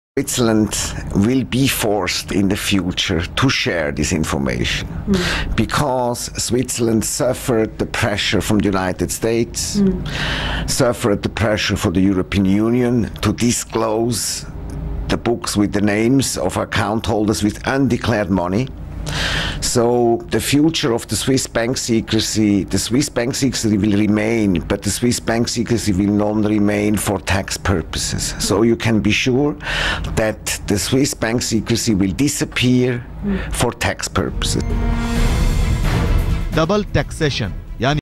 Enzo Caputo’s Interview on Secret Accounts of Indians 1
Switzerland will implement the automatic exchange of information according to article 26 of the OECD - Double Taxation Treaties starting most probably in 2018. That means that the Swiss Banking Secrecy for tax purposes will stop to exist in the near future.